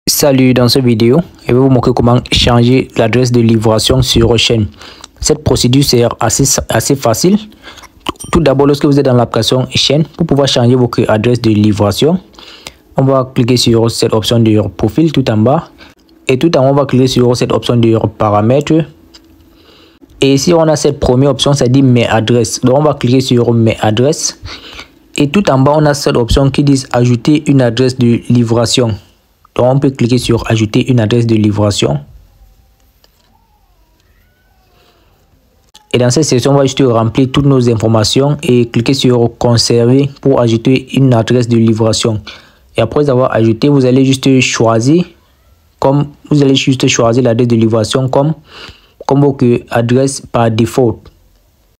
Comment Changer L'adresse De Livraison Sur Shein (Très Facile)
Dans cette vidéo, je vous montre Comment Changer L'adresse De Livraison Sur Shein (Très Facile)